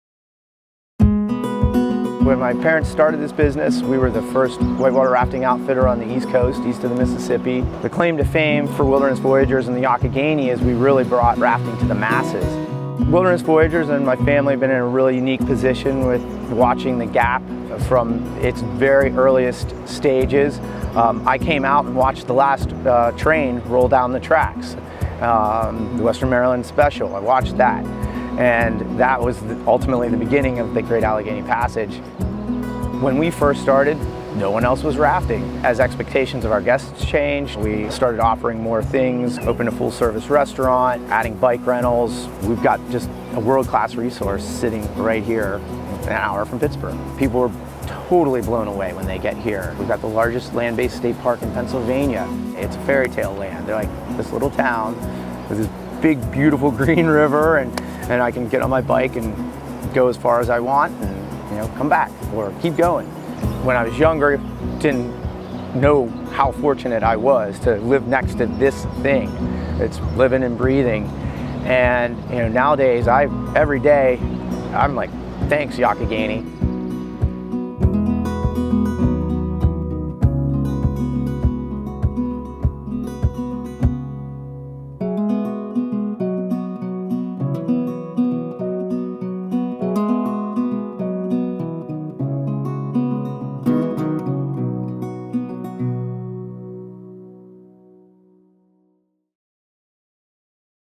Eric Martin -- Wilderness Voyageurs -- Great Allegheny Passage
Few people have navigated through the Youghiogheny River's Class IV and V rapids more often than Eric Martin, owner of Wilderness Voyageurs in Ohiopyle, Pennsylvania.  He grew up on this section of whitewater, and now runs one of the most successful outfitters and tour companies in the country.  Watch to find out why he likes pedaling the Great Allegheny Passage.